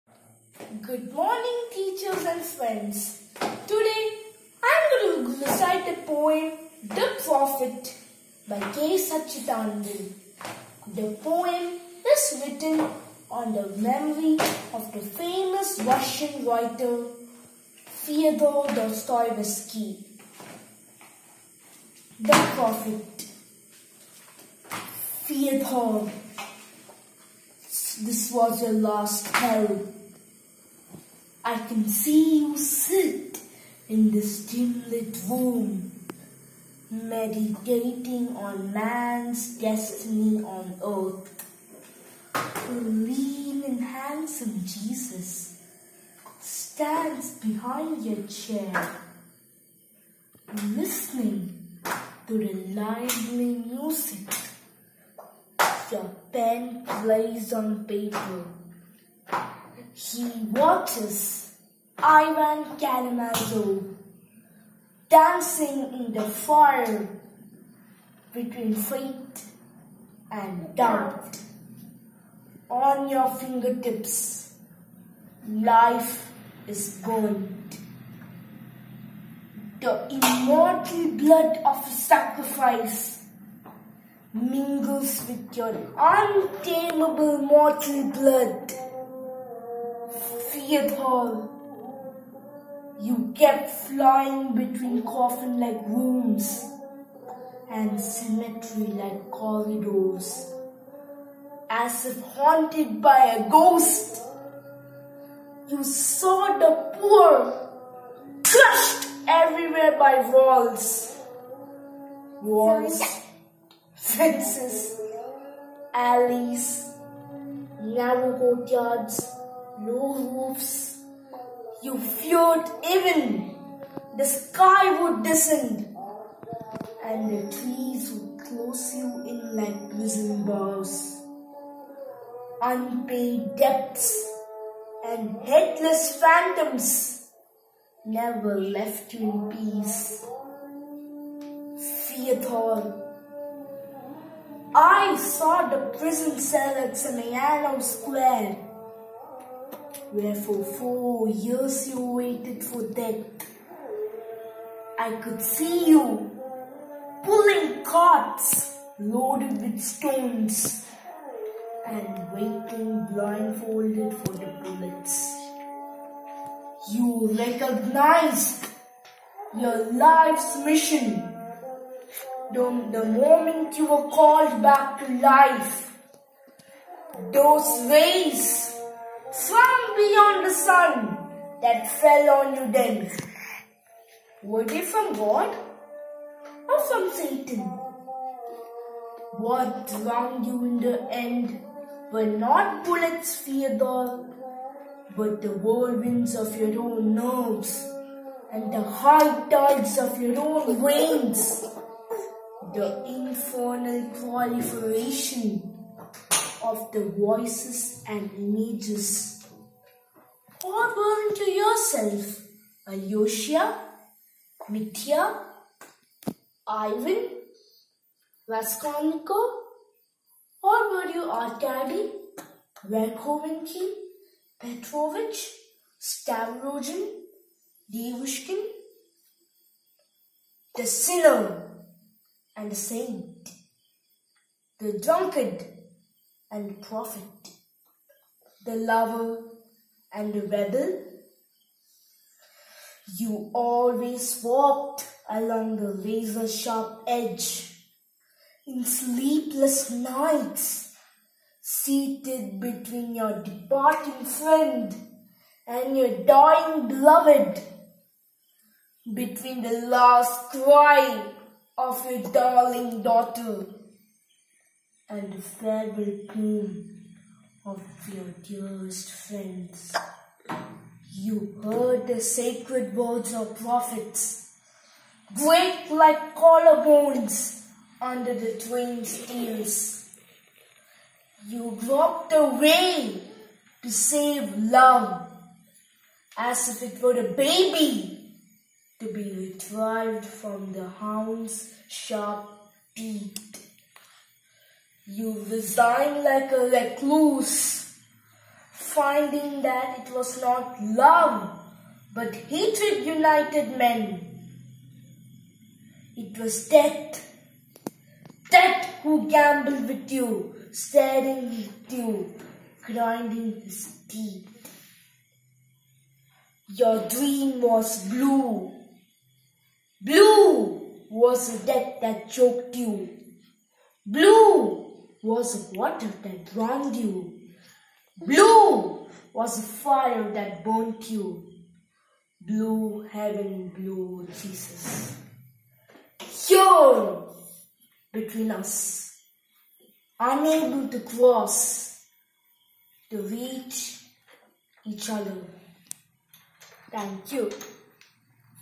The prophet, K.Satchidanandan
The prophet, a poem by K.Satchidanandan recited by Naethan.G.Rahul, Vidyodaya School, Ernakulam.